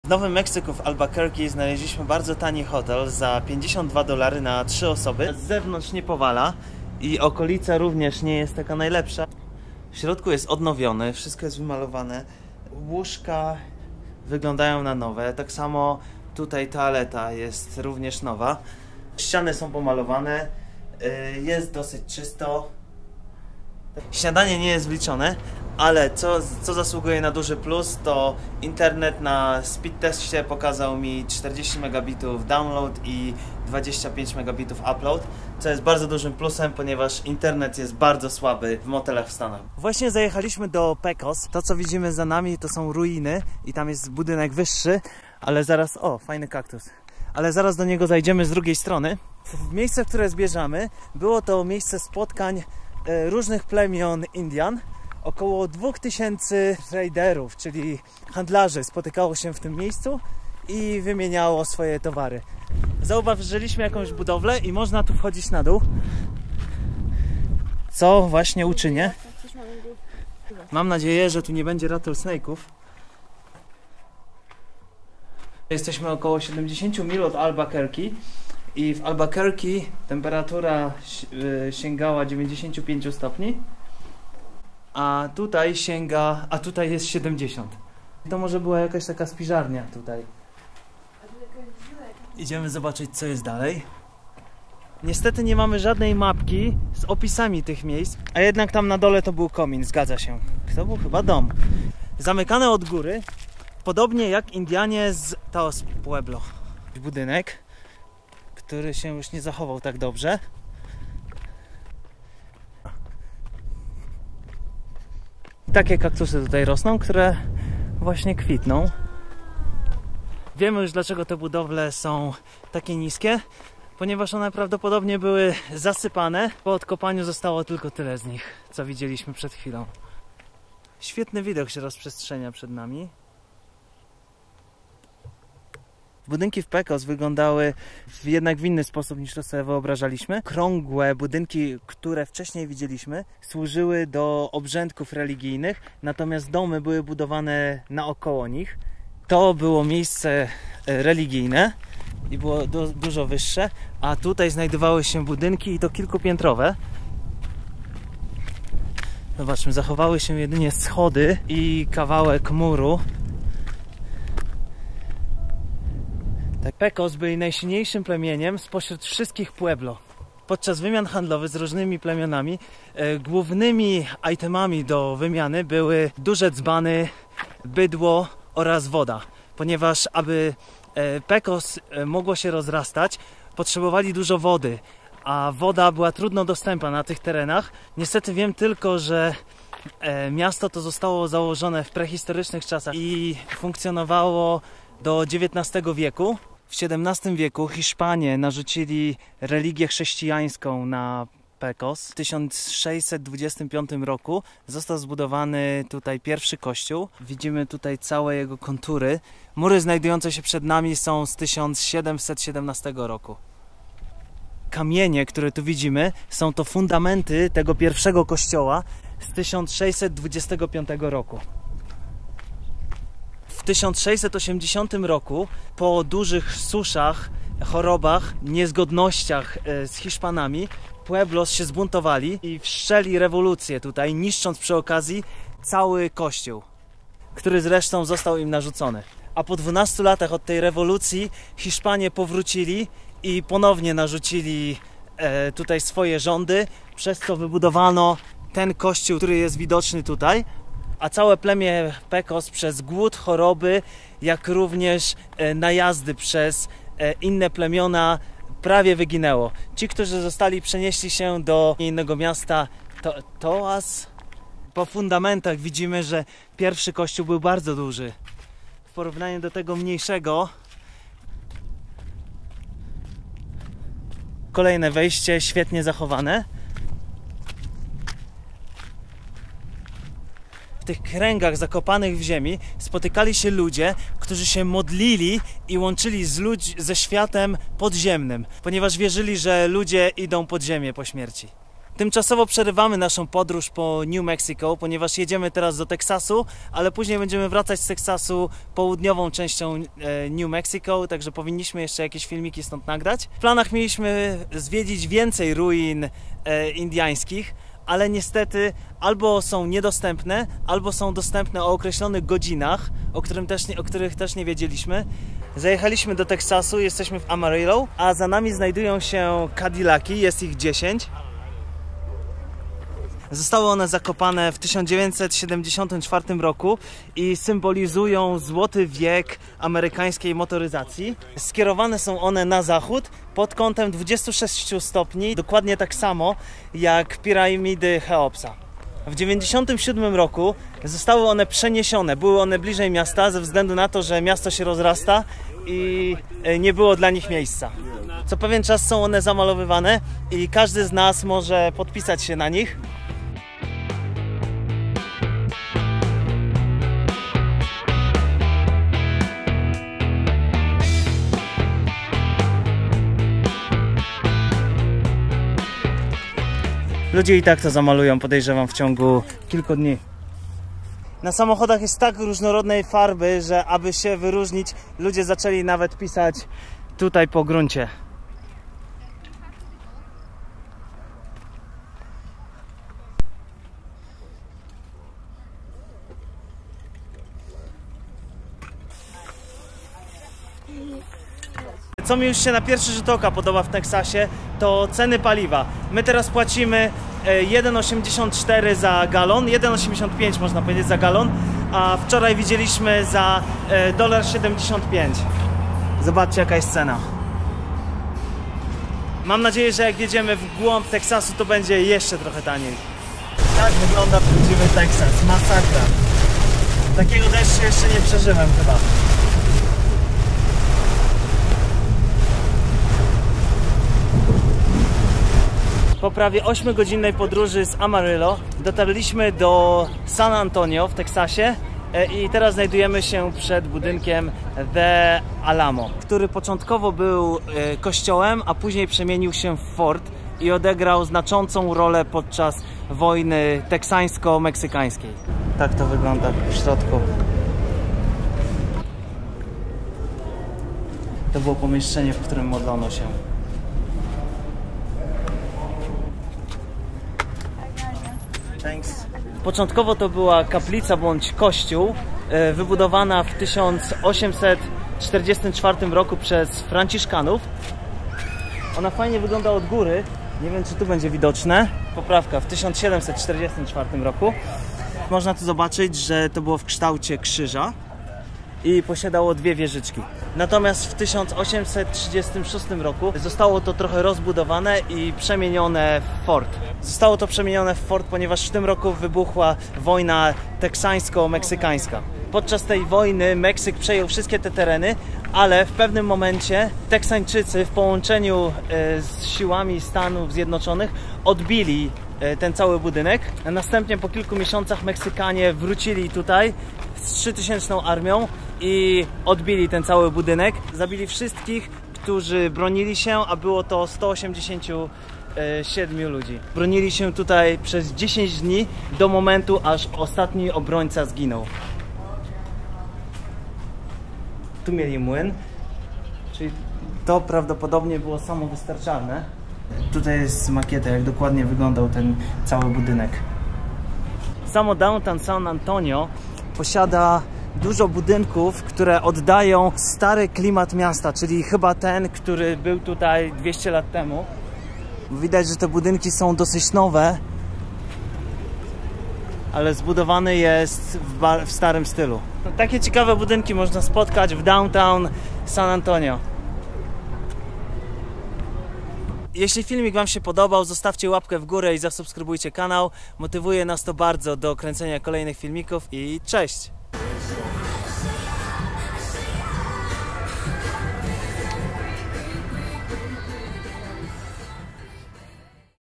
Ruiny Indiańskiego Pecos, Cadillac Ranch oraz The Alamo w Teksasie
Odwiedzamy ruiny indiańskiej wioski Pecos i chwilowo wstrzymujemy podróż po Nowym Meksyku. W Teksasie odwiedzamy 'cmentarz' samochodów Cadillac, zostawiamy po sobie znać i wjeżdżamy na największą ulewę naszego życia. Po 8 godzinach podróży dojeżdżamy do San Antonio i tam zwiedzamy The Alamo - fort/kościół.
Zapraszamy do oglądania :-)! Daje nam to motywację do kręcenia kolejnych materiałów.